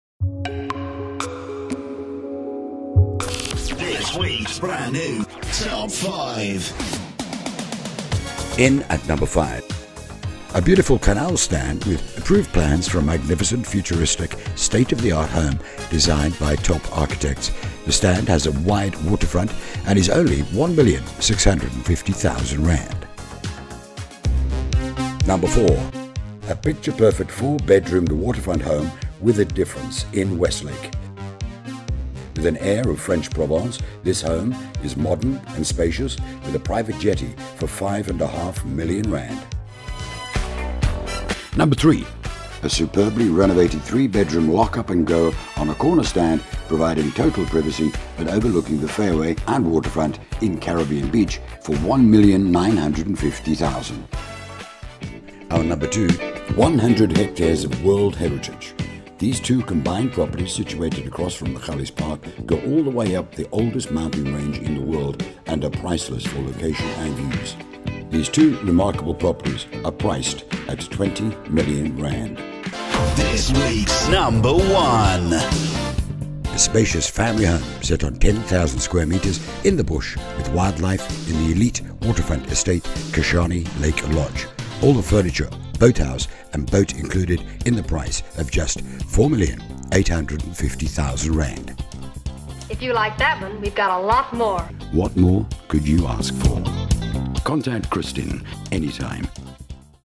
Top 5 Hartbeespoort Homes on Property 24 for 16 July 2023
Our top5 milliondollarlisting are

No 5 - EAST FACING CANAL STAND WITH WIDE WATER FRONTAGE. LAST ONE AT THIS PRICE. PLANS INCLUDED.

This stand is on a canal with a large waterfront and well priced. It comes with approved plans for a magnificent, futuristic state-of-the-art home designed by top architects.

No 4 - Picture perfect 4 bedroomed waterfront home in Westlake

This unique home has an aire of French Provence with it’s design and the Asiatic Jasmyn climber which covers much of the homes façade.

This spacious open plan home has an abundance of light and is beautifully set on the banks of a private inlet to Hartbeespoort Dam.

The vast downstairs encompasses an impressive double volume entrance, 2 lounge areas, dining room and modern kitchen all facing the patio with a sparkling swimming pool and sunken trampoline. The private jetty has a floating dock for a boat and a jet ski. The waterfront has sprays for constant circulation.

All 4 bedrooms are spacious with lush carpets, TV points and air conditioned. There are 2 identical, massive main en-suites both with lounges, facing the dam.

The bathrooms are modern with travertine tiles.

Staff accommodation and three garages complete this lovely family home  for just R 5.5 Million. Furniture and appliances, boat and jetski optional.

No 3 - SUPERBLY RENOVATED AND FABULOUSLY DECORATED LOCK UP AND GO

his totally private unit is situated on the last corner of the estate, sheltered by the most impressive Carob tree overlooking the fairway and the dam.

The renovation is flawless from the top end Teracoat light grey screed floors, through to the designer tile features to trendy lighting and expensive seamless stacking doors; unsurpassed elegance.

The stylish kitchen has Samsung appliances and space for both dishwasher and washing machine. The closed in patio has a braai and mini kitchen and the stacking doors lead onto a composite decked outdoor lounging and dining area with the backdrop of the most overwhelming views for the surrounding mountains, fairway and dam.

All 3 bedrooms are ensuite and the 2 upstairs ones have balconies. Every room is air conditioned. Furniture is optional.

No 2 - 100 HECTARES OF WORLD HERITAGE.

These 2 combined properties situated across from Magalies Park go all the way up the oldest mountain range in the world and are priceless for location and views.

The first property is 50 Hectares and has an Authentic African designer home with breathtaking views of the dam and surrounded by wildlife.
The main dwelling is spacious and comprises 2 en-suite bedrooms and an office, a massive circular lounge with an open fireplace leading onto a truly “Out of Africa” veranda with views of the dam.

A separate cottage offers 3 bedrooms, 2 bathrooms, an open plan lounge and kitchen and a patio.
Then there are 4 garages, a huge storeroom, staff quarters and a laundry house.

The property is fully fenced and has roaming game including Kudu, Wildebeest, Impala, Water buck to name a few species. It also has 2 large green house tunnels and a massive multi purpose congregational structure.

The second property measures 50 hectares. This land is commercialy zoned and has approved plans for a wedding venue, restaurant, Spa and lodges. With a separate entrance off the main road, this property also reaches all the way to the top of the Magaliesberg Mountain range. With magnificent views of the dam and the surrounding nature reserves. This property is ideal for a tourist retreat, fine dining venue, health spa or a rehabilitation centre. The property is also suitable for a family dwelling. There are no land claims.

These two remarkable properties are priced at R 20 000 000.

No 1 -  Spacious single storey family home in K’Shane Estate.

This home is a full package and includes furniture, a boathouse and a boat. It is superbly built with quality finishes throughout.

Set on 10 000 m2 stand with river frontage, the manicured lawns lead to a boma and play pen. The home has it’s own water tanks, a generator and fibre.

The boat is a 10 seater outboard with a 125 HB engine with very low mileage. Water toys included.

All this for just R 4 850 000